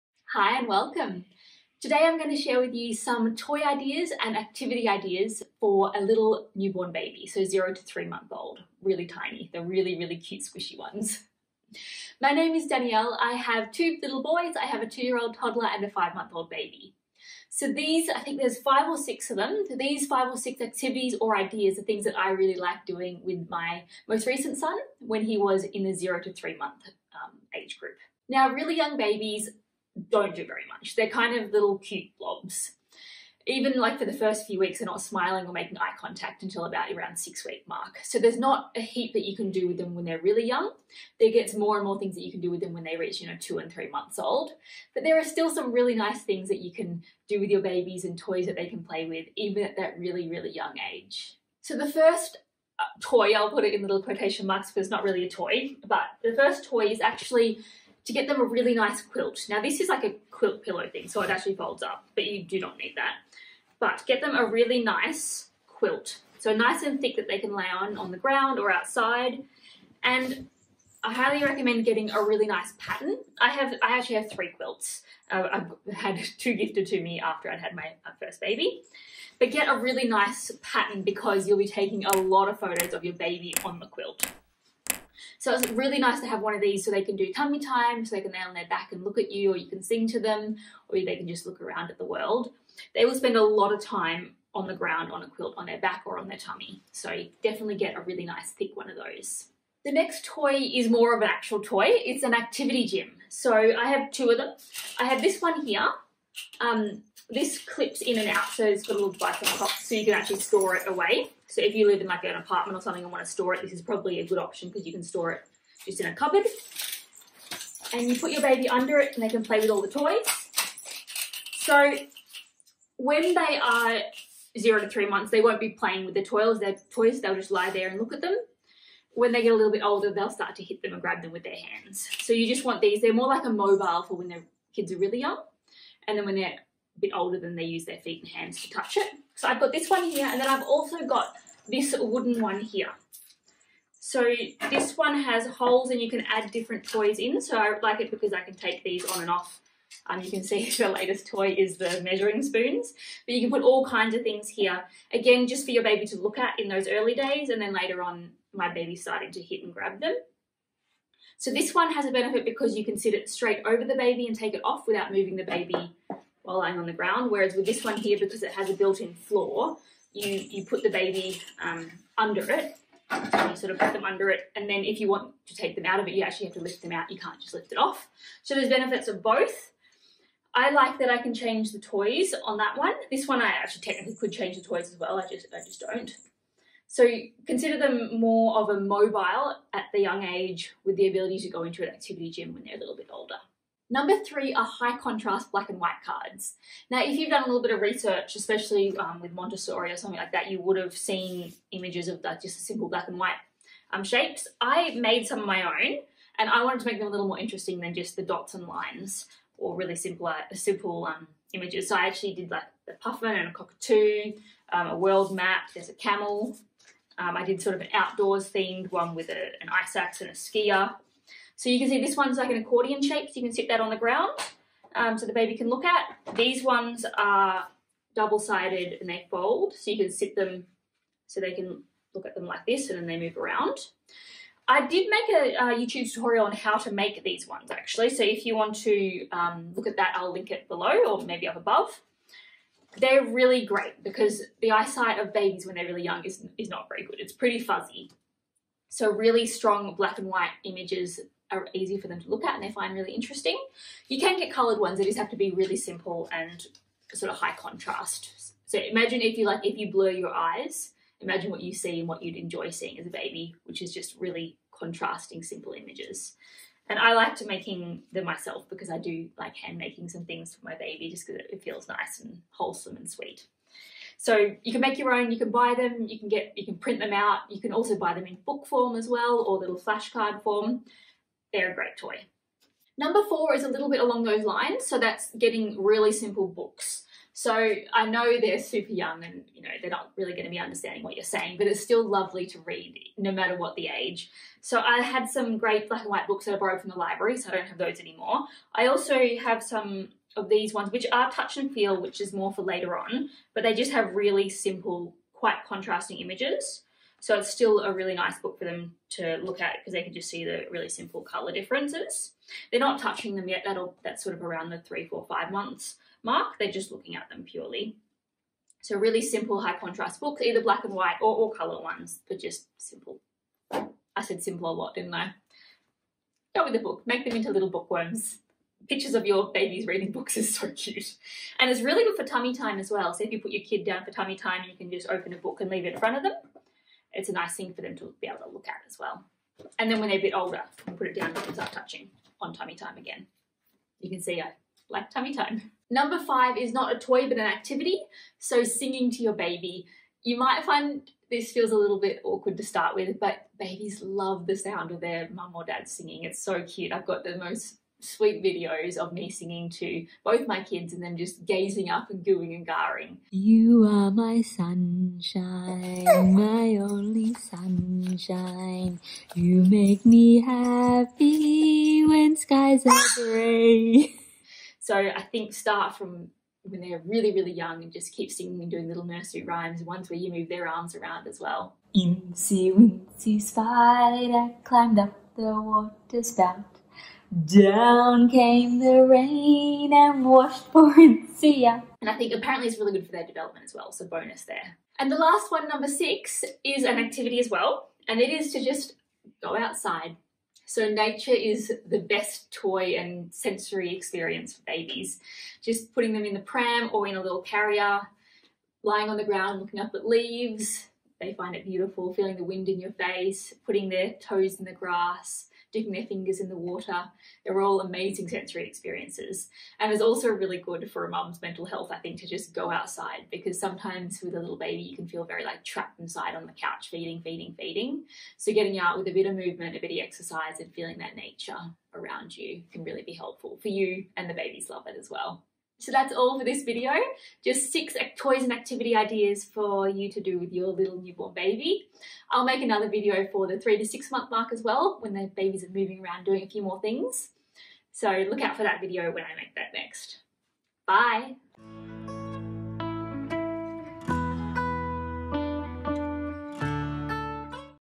6 NEWBORN toys: Best TOYS & ACTIVITIES for 0-3 months
This video is my top 6 newborn toys and activities. It is all about the best toys for the 0-3 month baby age bracket. These are the toys I have loved using with my little newborn baby. If you are expecting a baby soon or if you have a newborn baby in the zero to 3 month age group then you will hopefully find some fun ideas to try in this video. From quilts, activity gyms, high contrast cards, singing and going outside.

CONTENT
00.00 Introduction
00:51 Quilt
01:40 Activity Gym
03:13 High Contrast Cards
04:42 Books
05:45 Singing
06:34 Nature